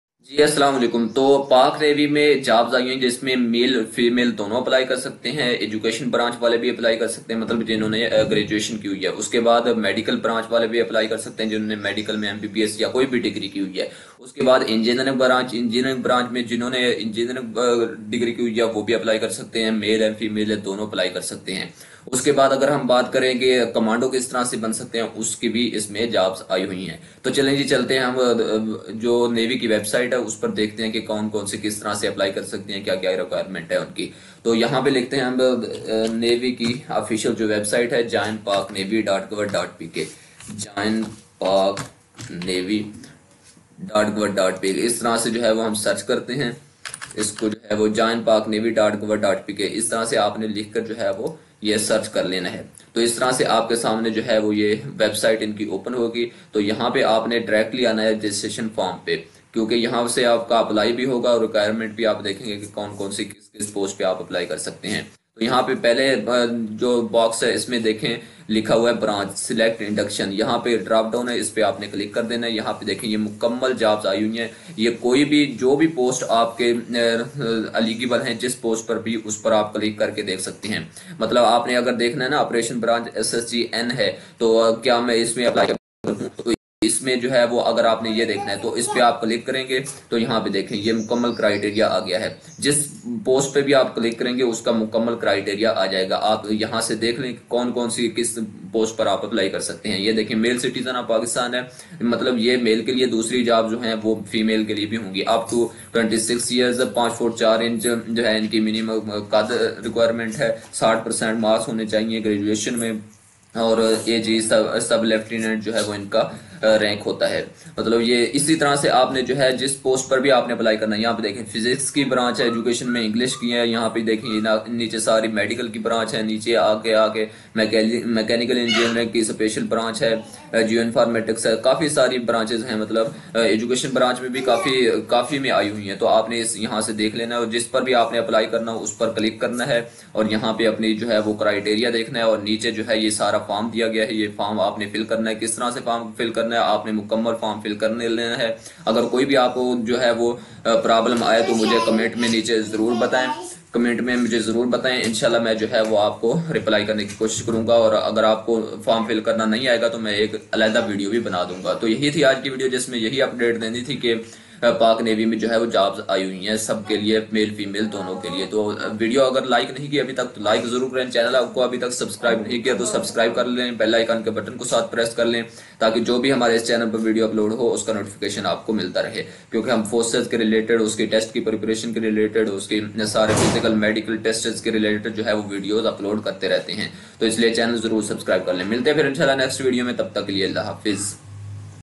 Pak Navy Edu, Med, Eng branch & SSG Commando jobs 2022 | Pak Navy SSC new jobs 2022
Yes Assalamualaikum!

In this video, I'm going to discuss about Pak Navy SSC new jobs 2022. There are a lot of posts are announced by Pak Navy in which graduate students can apply in different branch both male and female..

In following catagarise, Pak Navy has announced jobs;

☑️ Education branch jobs
Physics, chemistry, math, IT, Islamiyat, Pak Study etc.
☑️ Medical branches jobs
GDMOs and many more.
☑️ Engineering branches jobs
Mechanical engineering, aviation branch jobs on navy.
☑️SSG commando jobs in navy

What are the eligible criteria of these jobs and how to apply in Pak navy SSC jobs and other complete information is discussed in this short video...

Remind me in your prayers!

Your mentor: Ch M Umar

Channel Name: Pak Forces Info